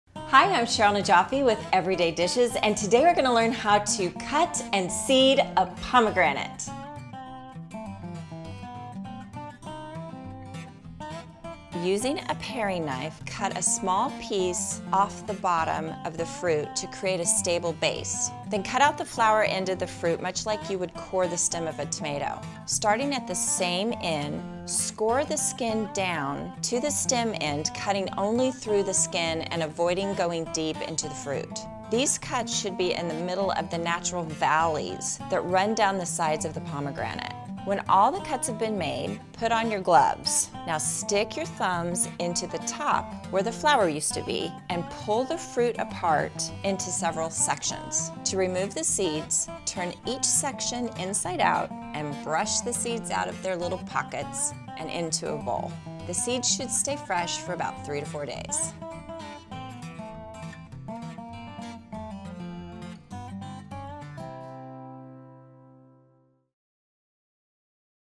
How to Seed Pomegranates
Simple, easy, and oh-so-fresh!

Ingredients:

1 pomegranate
Vinyl or latex gloves